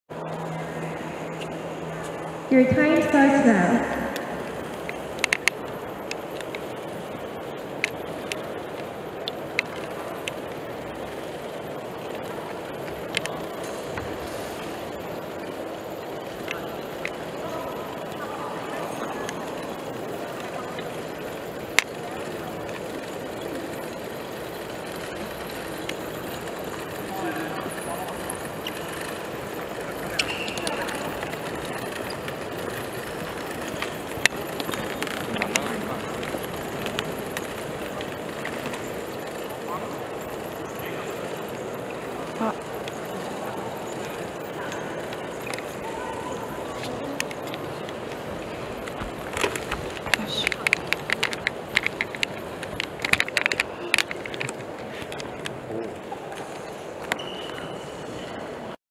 KAIST Chem-E-Car Team to Vie for World Title
Team KAItalyst, composed of KAIST undergraduate students, celebrated victory in the regional qualifying rounds of the 2019 International Chem-E-Car Competition held at KAIST’s Main Campus in Daejeon on July 20. The high finish in the national rankings qualified the team for a trip to the world finals to be held in Orlando, Florida, USA, in November.